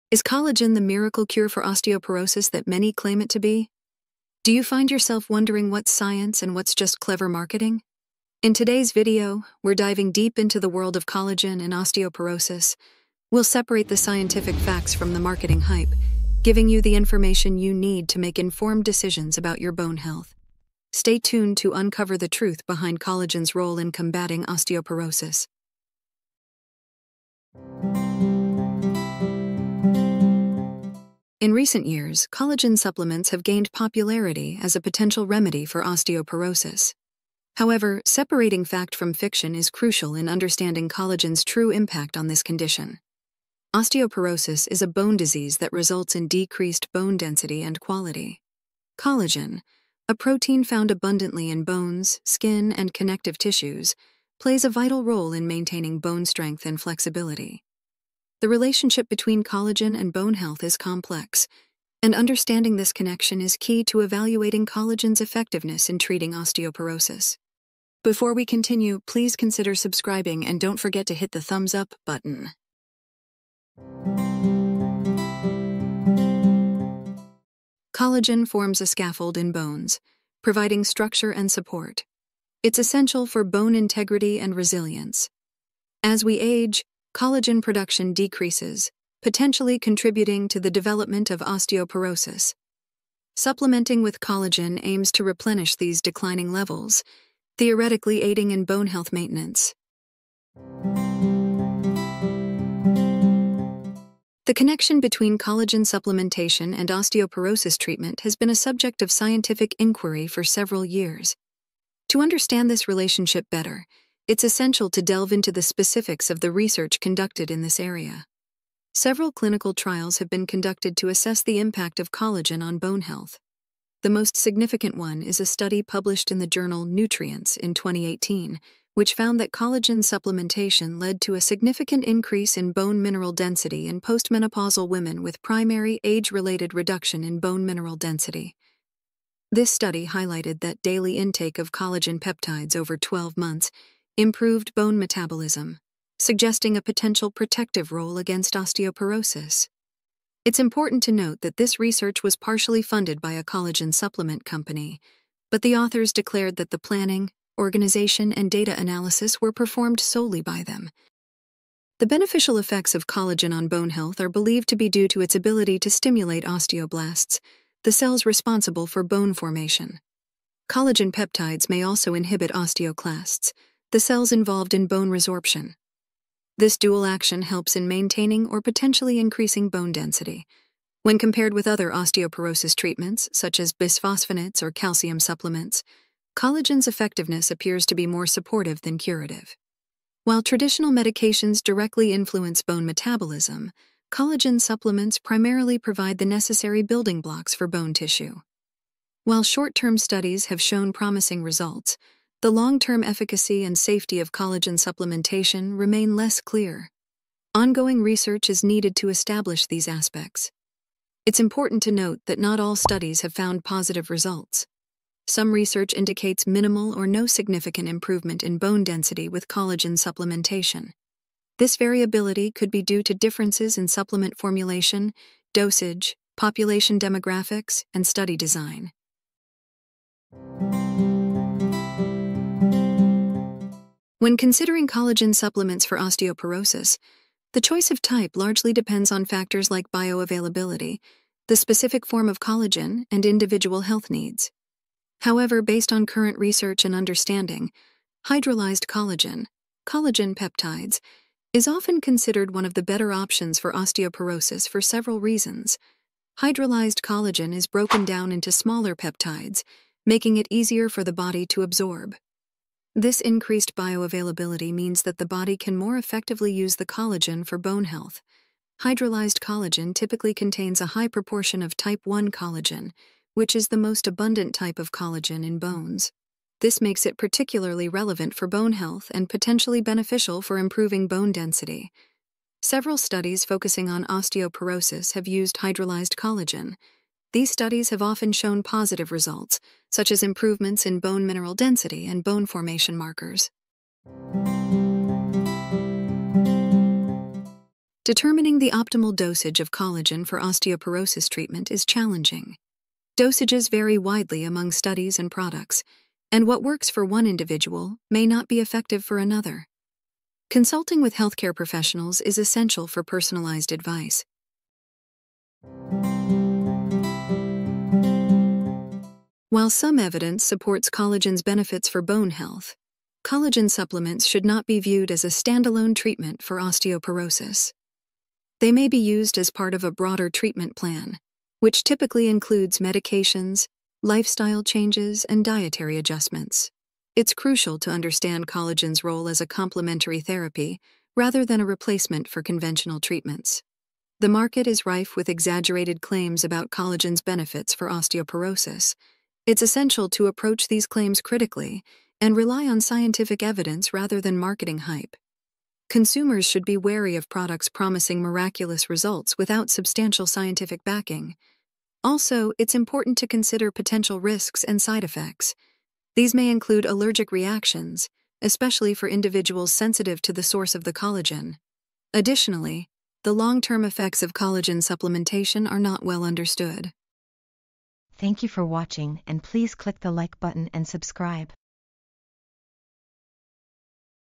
Collagen for Osteoporosis: Scientific Facts vs. Marketing Hype
Are you tired of sifting through endless claims about collagen supplements and their supposed benefits for osteoporosis? Do you find yourself wondering what's science and what's just clever marketing? In today's video, we're diving deep into the world of collagen and osteoporosis. We'll separate the scientific facts from the marketing hype, giving you the information you need to make informed decisions about your bone health.

🌟 TIMESTAMPS 🌟
Introduction 0:00
Understanding Osteoporosis and Collagen 0:27
The Role of Collagen in Bone Health 1:13
Scientific Evidence on Collagen and Osteoporosis 1:40
What Type of Collagen to Take 3:50
Collagen Dosage and Administration 4:53
Should you take collagen for osteoporosis? 5:15